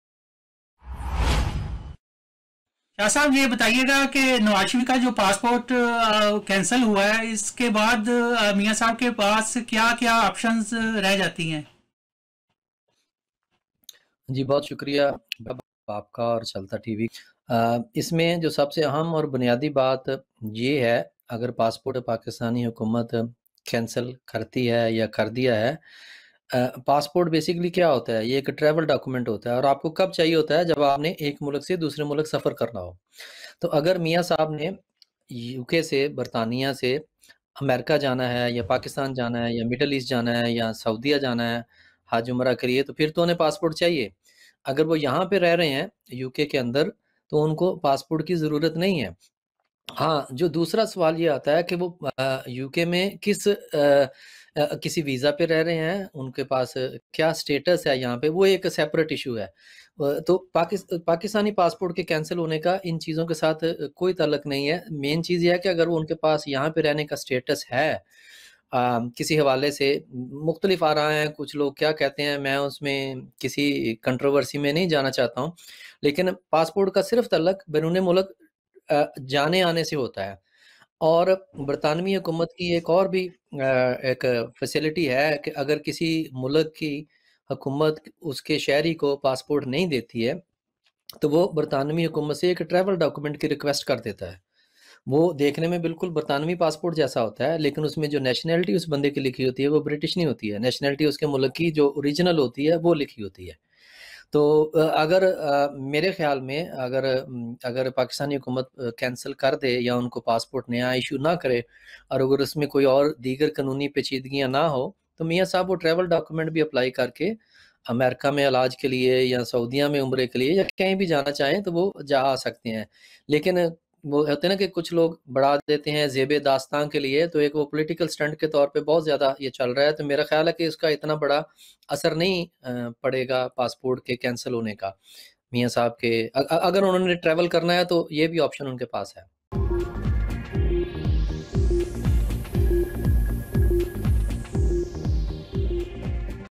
Nawaz Sharif has a lot of options - passport cancellation won't matter
نواز شریف کے پاس کافی آپشن موجود ہیں - پاسپورٹ کینسل ہونے سے کوئی فرق نہیں پڑے گا